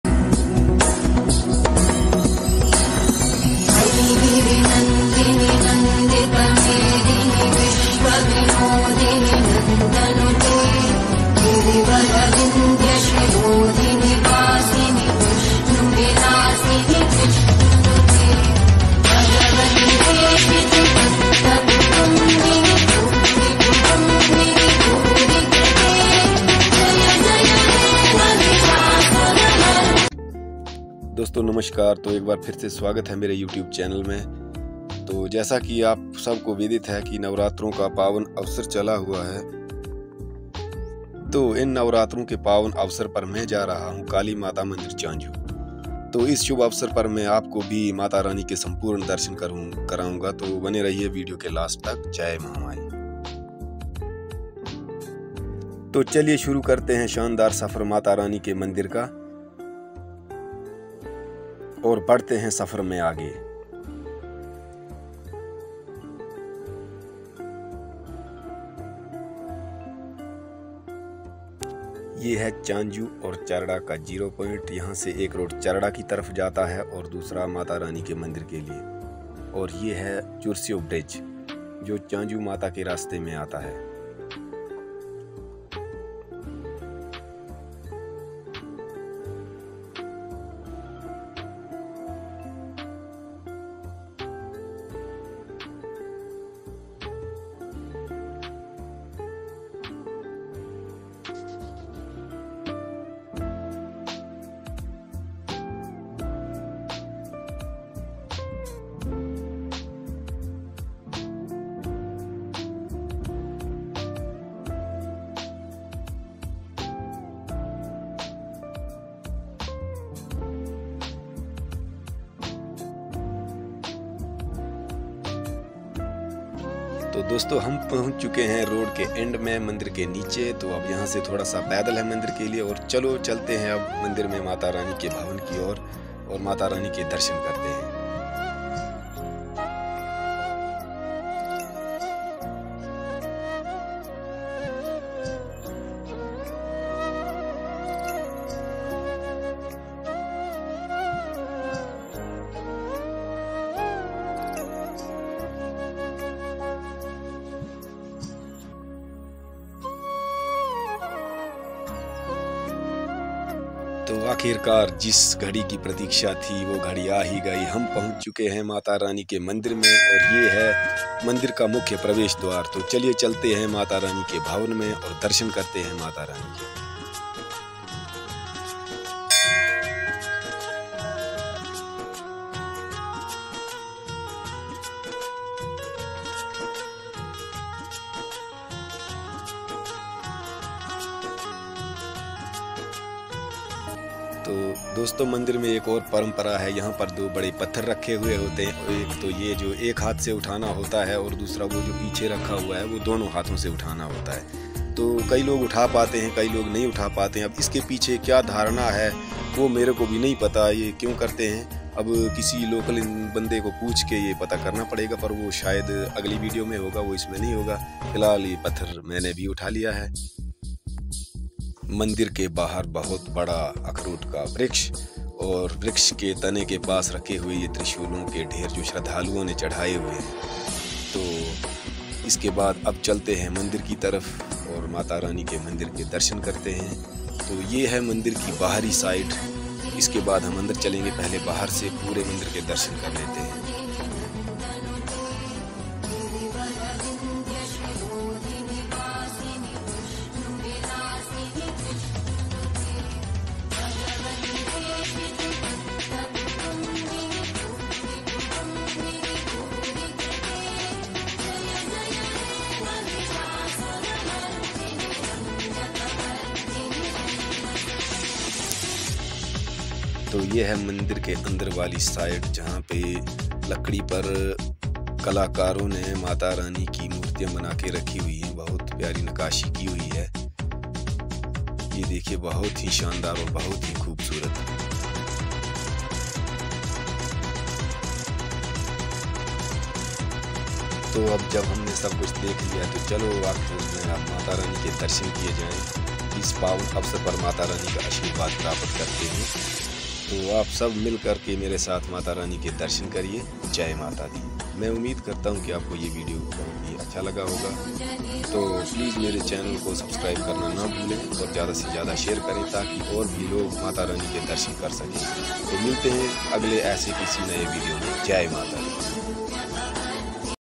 Chanju Mata Darshan ।। चांजू वाली काली माता के दर्शन।। नवरात्री विशेष।।
नवरात्री के पावन अवसर पर काली माता चांजू वाली के मंदिर की यात्रा माता रानी की असीम कृपा से माता रानी के दर्शनों का सौभाग्य प्राप्त हुआ
इस वीडियो के माध्यम से आप सब भी माता रानी का आशीर्वाद प्राप्त करे